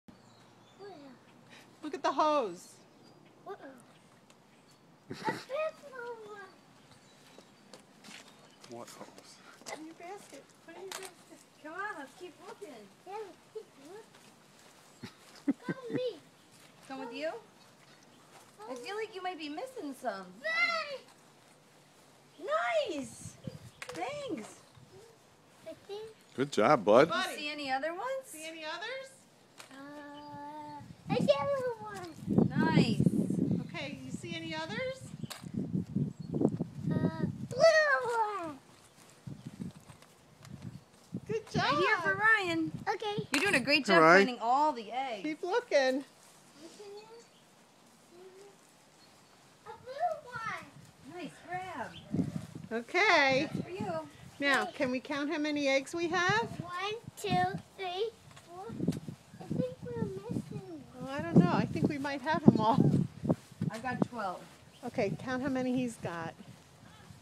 Easter egg hunt
Henry on the hunt for eggs in buffalo